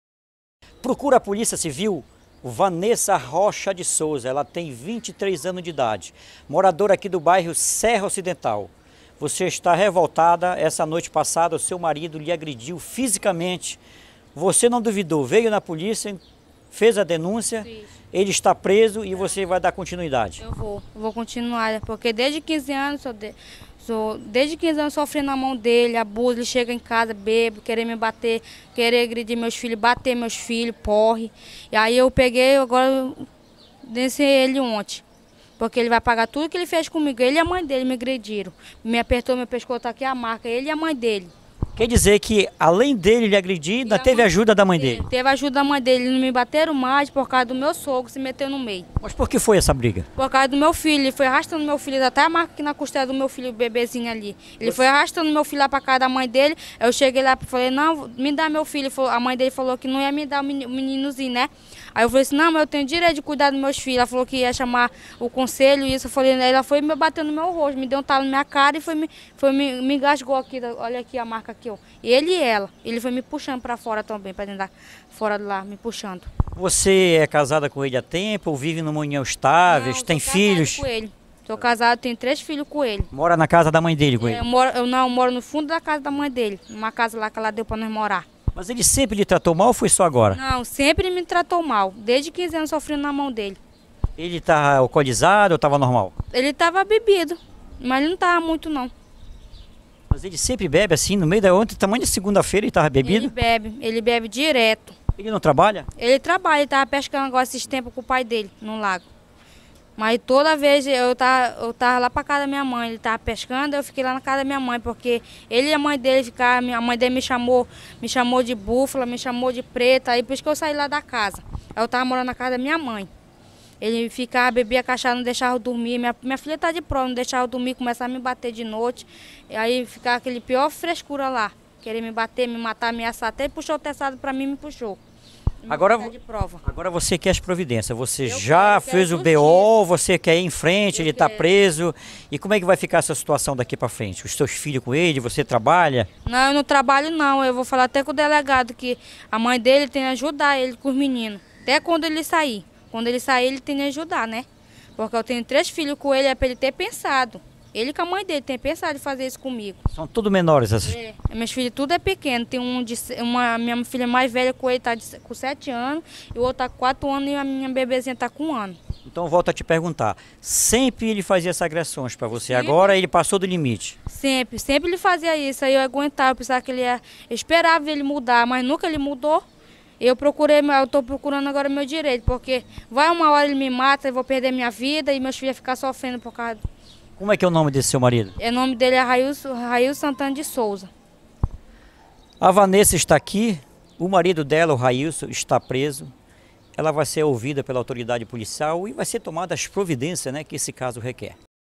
Mulher denuncia marido por violência Domestica
Mulher denuncia marido por violência Domestica / Monte Alegre, Pará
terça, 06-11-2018